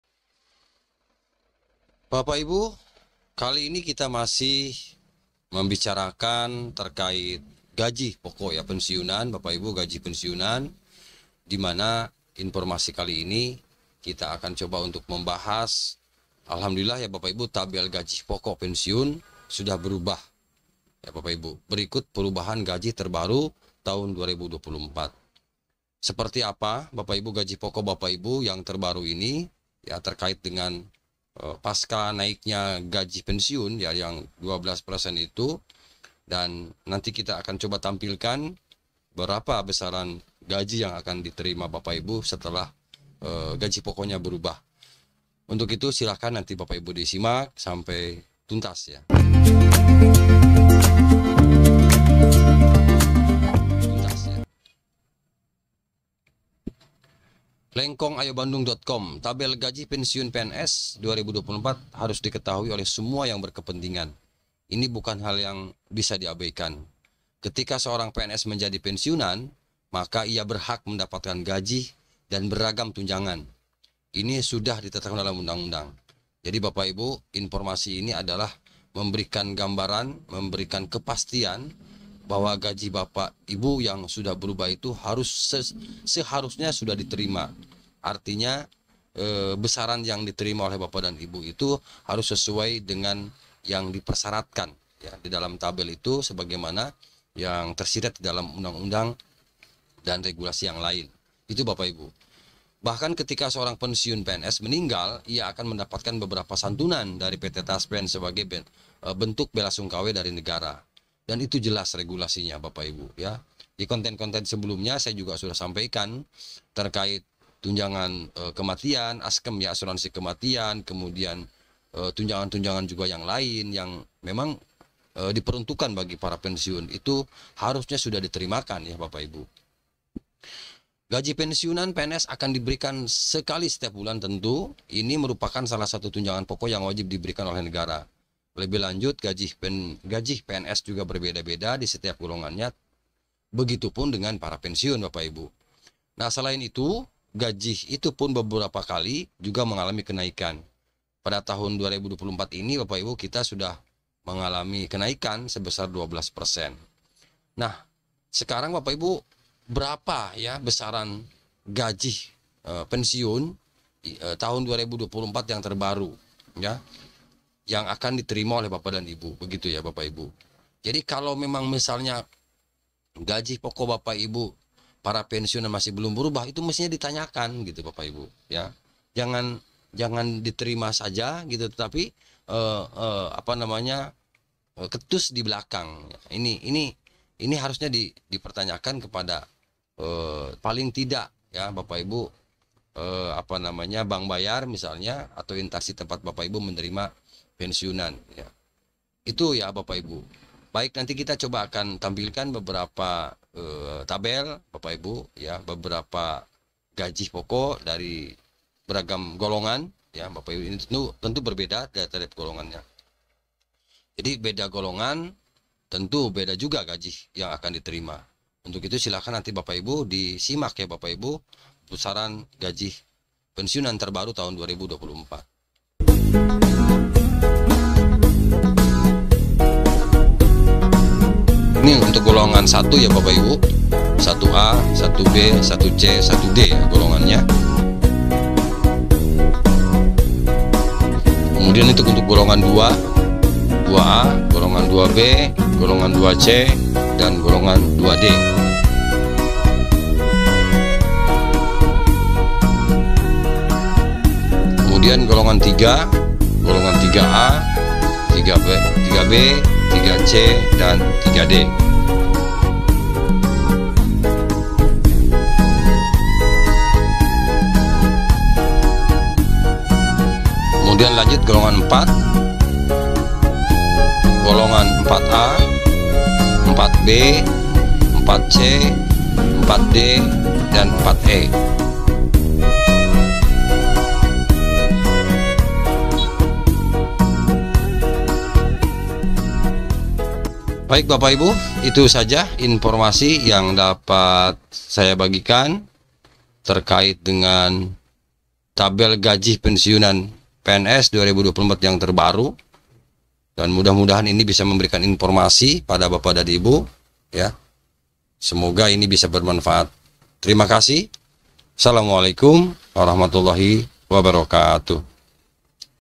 Tabel Gaji Pensiunan Sudah Berubah || Berikut Perubahan Gaji Terbaru Tahun 2024
Assalamualaikum wr.wb
Bpk/Ibu informasi yang akan admin sajikan adalah terkait gaji pokok yang mengalami perubahan, hal ini dampak dari kenaikan gaji pensiunan sebesar 12%. Tentnya dengan kenaikan gaji tersebut berpengaruh terhadap penghasilan para pensiunan. Selengkapnya silahkan disimak sampai selesai  @kangedibae